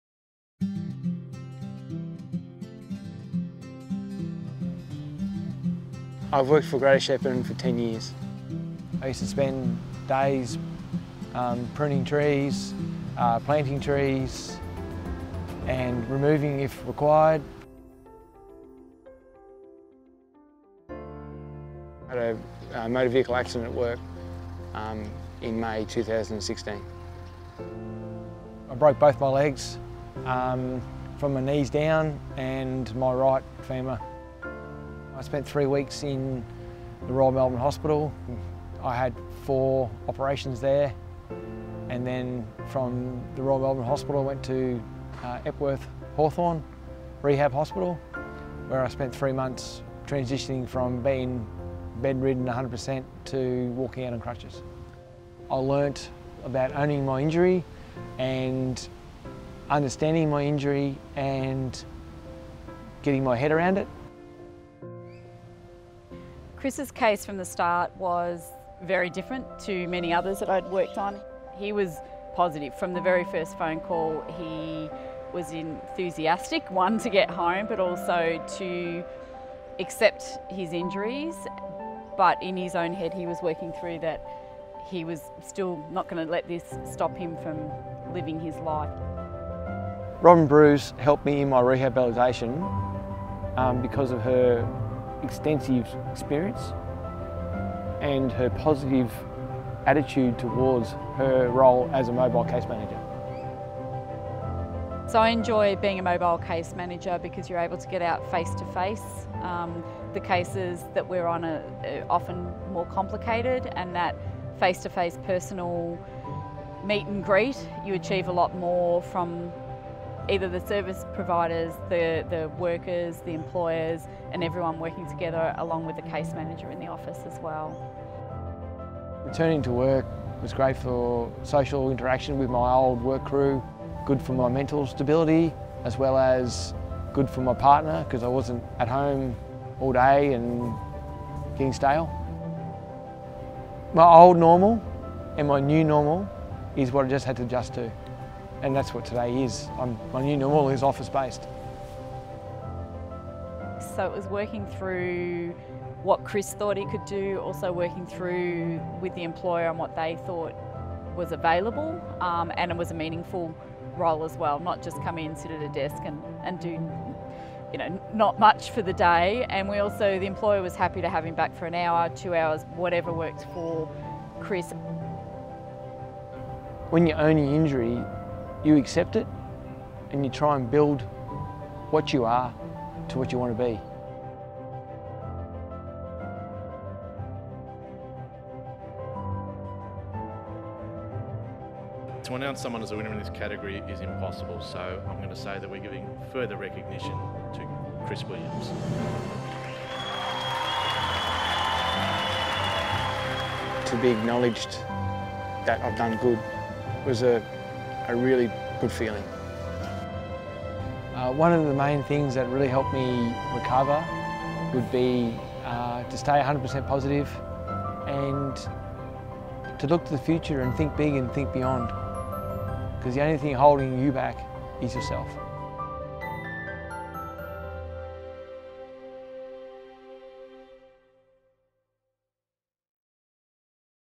Owning your injury – Chris and Robyn’s story
Chris’s life changed in an instant when he was involved in a serious car accident while on the job as an arborist. He suffered multiple injuries and had to undergo numerous surgeries – it’s been a long road to recovery. Chris talks about the support he received from his colleagues, his family and his case manager, Robyn, to get back to work and adjust to a new normal as a mainly office-based worker.